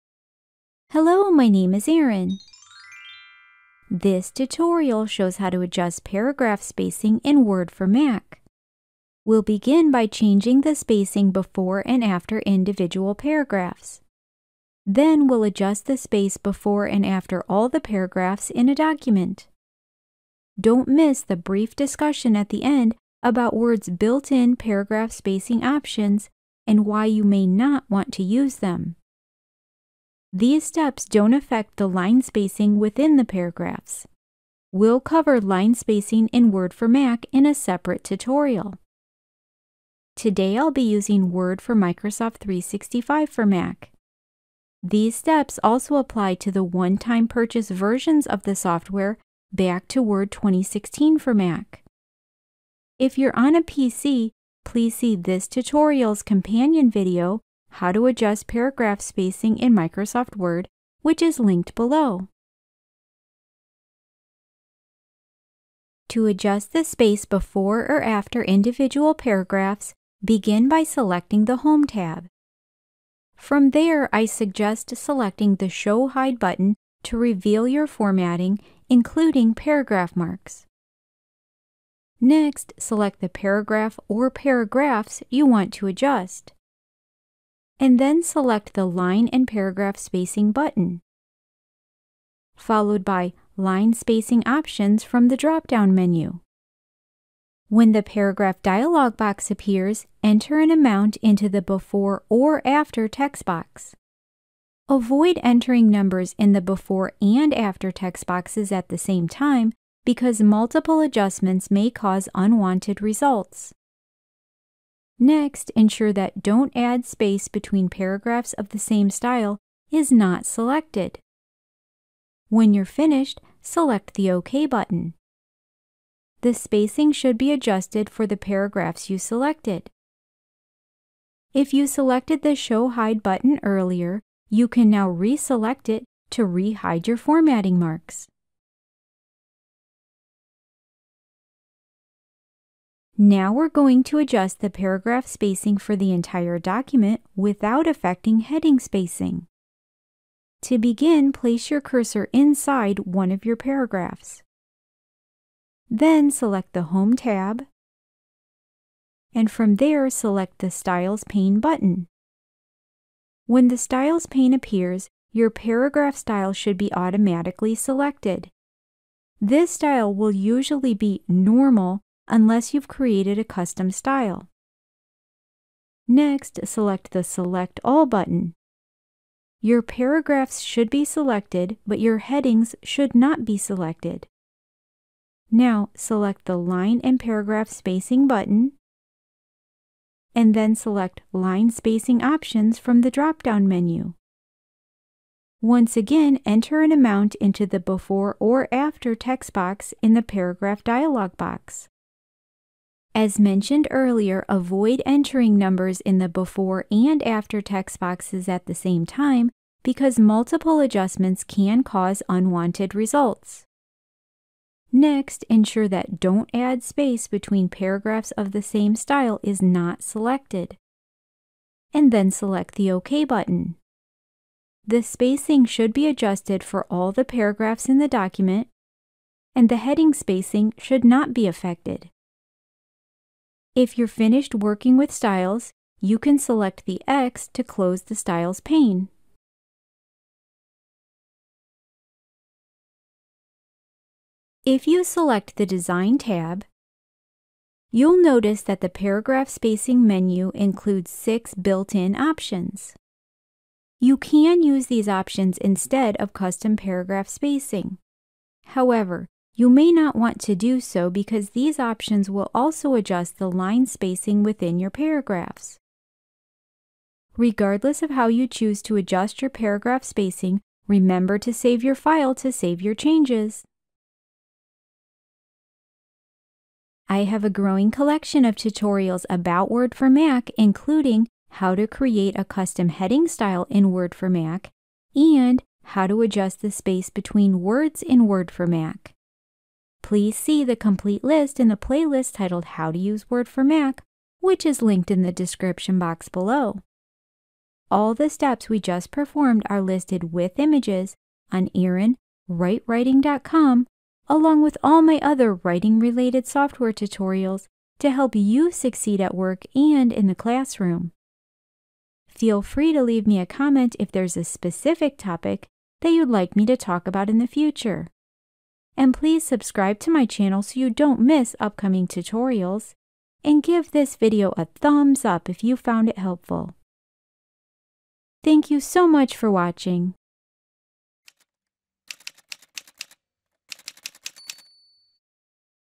How to Adjust Paragraph Spacing in Word for Mac
Learn how to adjust paragraph spacing in Word for Mac including the spacing before and after individual paragraphs and the entire document.

These steps apply to Word for Microsoft 365 for Mac, Word 2021 for Mac, Word 2019 for Mac, and Word 2016 for Mac.

Chapters:
00:00 Introduction
01:05 How to Adjust the Space Before and After Individual Paragraphs in Word for Mac
02:22 How to Adjust the Space Before and After All Paragraphs in Word for Mac
04:02 What about the Built-In Paragraph Spacing Options?

How to Add Page Numbers in Word for Mac